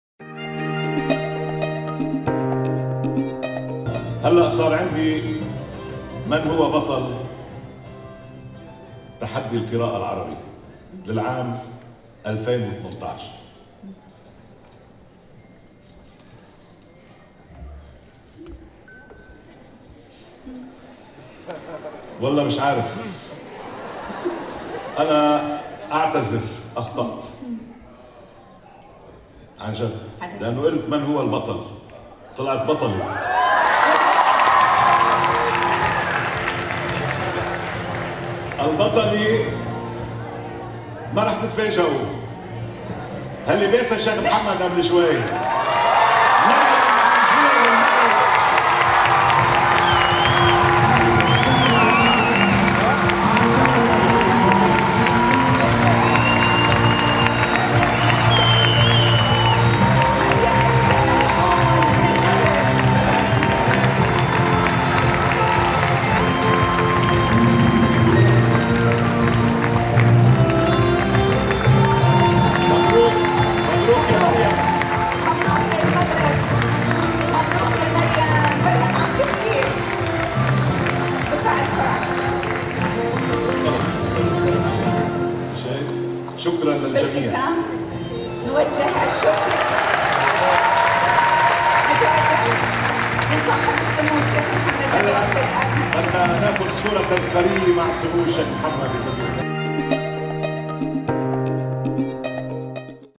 الشيخ محمد بن راشد ينحني تواضعا ويمسح دموع فرح الطفلة المغربية مريم أمجون
الشيخ محمد بن راشد ينحني تواضعا ويمسح دموع فرح الطفلة المغربية مريم أمجون بطل تحدي القراءة العربى